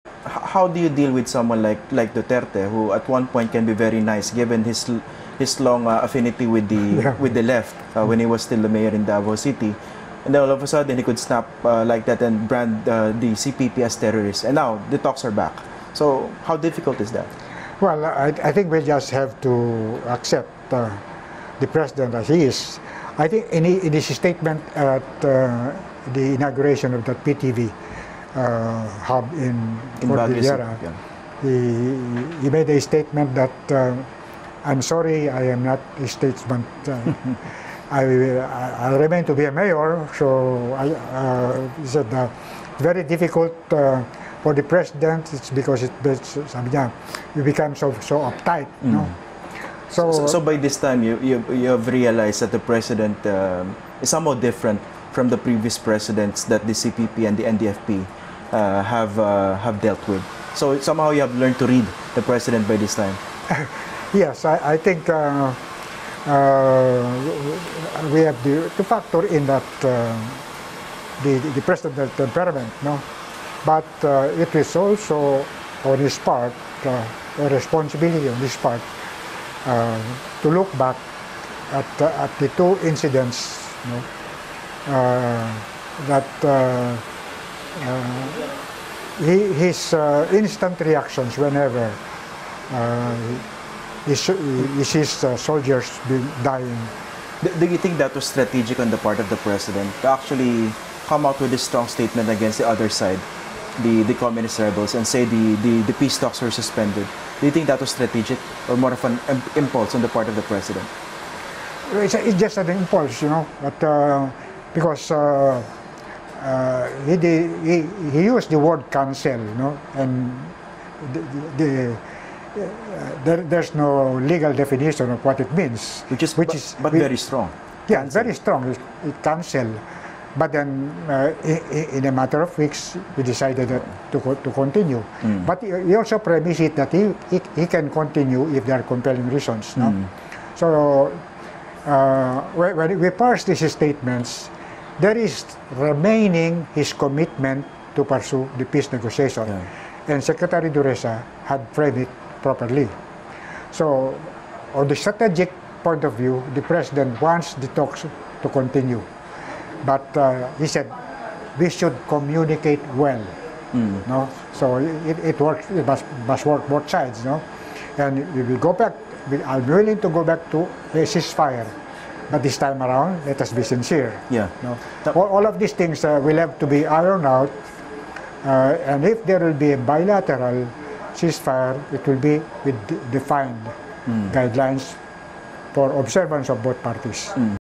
Satur Ocampo says rebels understand Duterte's moods
Former National Democratic Front negotiator Satur Ocampo on Monday said communist rebels have now gotten used to President Rodrigo Duterte's moods following the collapse and resumption of peace talks.